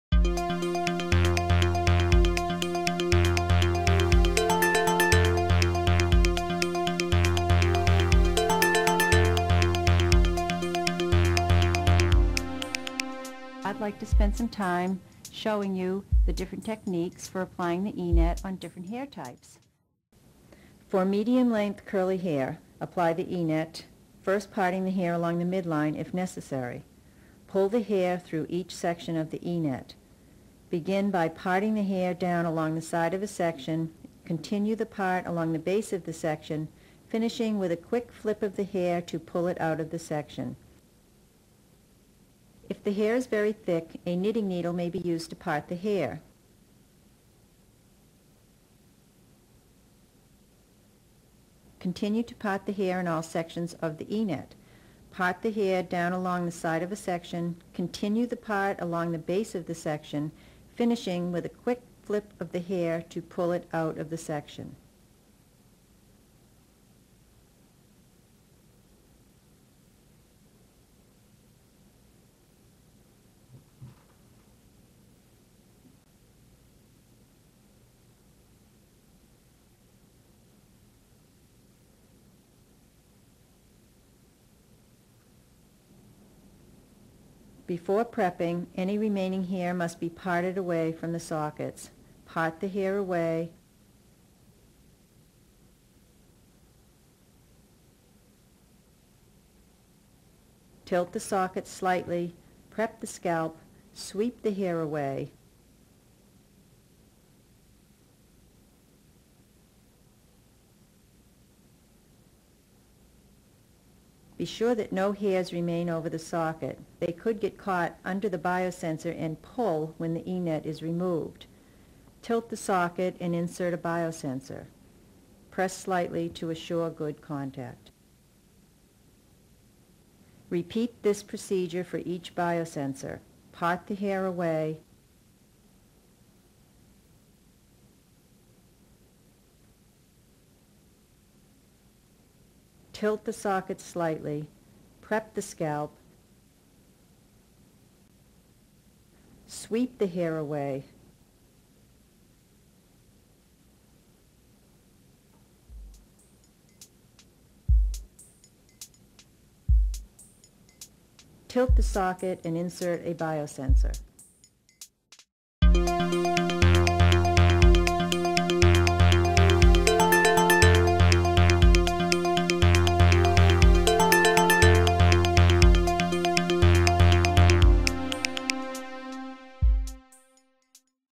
Epilepsy? EEG with HydroDot System Video 4 -Curly Hair
Epilepsy? EEG electrode application with The HydroDot® EEG Application System for recording brainwaves. This video shows use of the product on a patient with medium-length curly hair. Great for kids and adults. Reduces risk of cross-contamination and no messy clean-up!